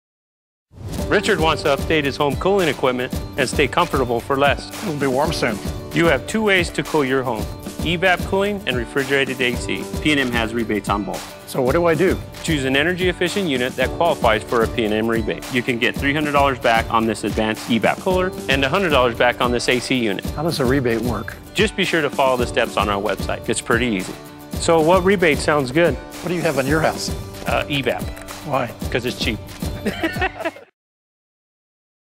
PNM Rebates for Cooling Upgrades
Learn more about rebates on upgrading your cooling equipment. Visit PNM.com/coolrebate for full details.